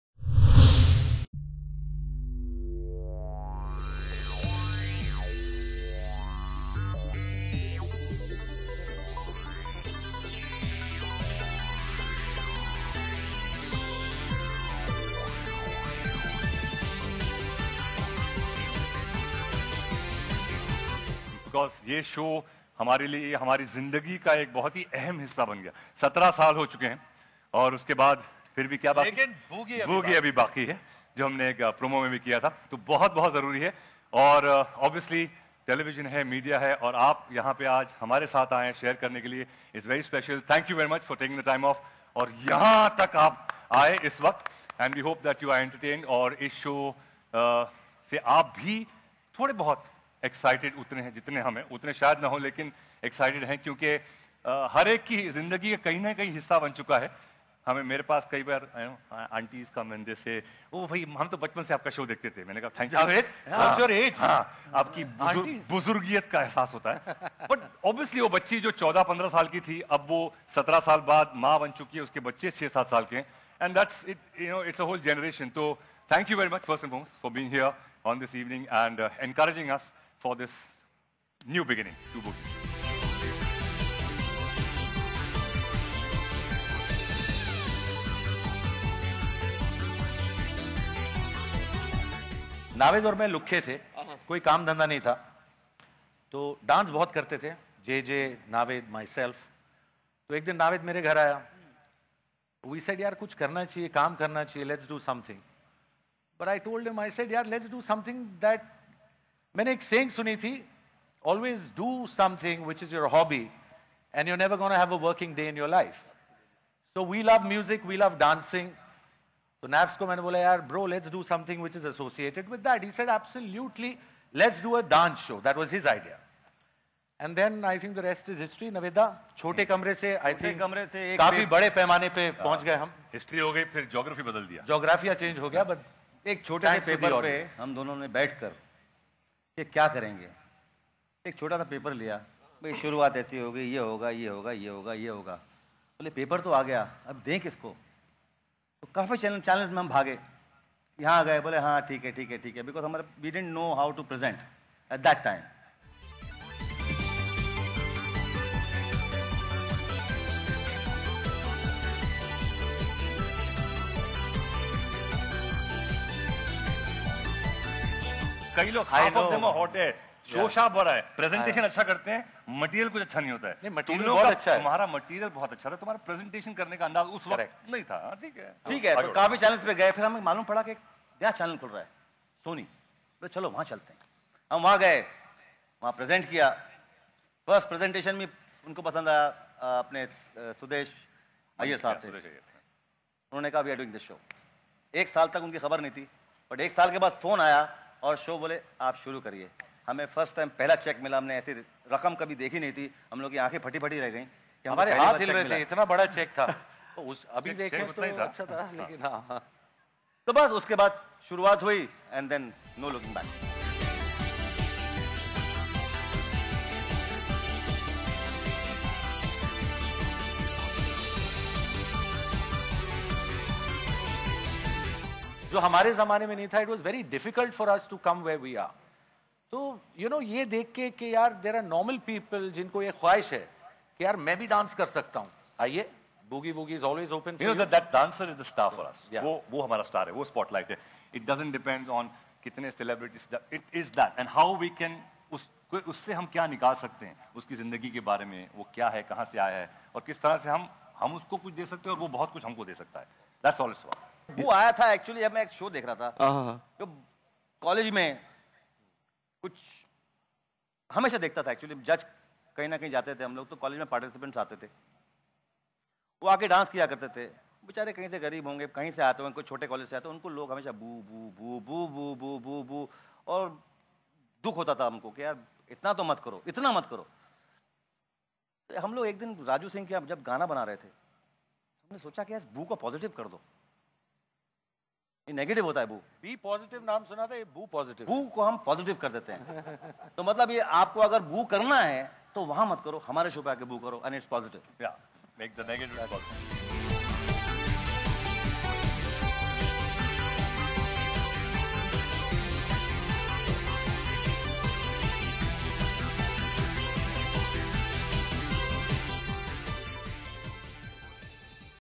Launch Of TV Show Boogie Woogie
Its time to bring back the fun, the entertainment and the humour into dancing, as Sony Entertainment Television is all set to create a fresh legacy of dance, with the return of India's original dance show, Boogie Woogie. The New Season starting December 7,2013 will redefine your weekends as some of the most talented young dancers from across the country compete for the title of Boogie Woogie Kids Championship. Produced By R & N Productions, Boogie Woogie Kids Championship brand new season will once again mark the return of the hilarious trio of Jaaved Jaferi, Naved Jafri and Ravi Behl who will be reprising their roles as judges.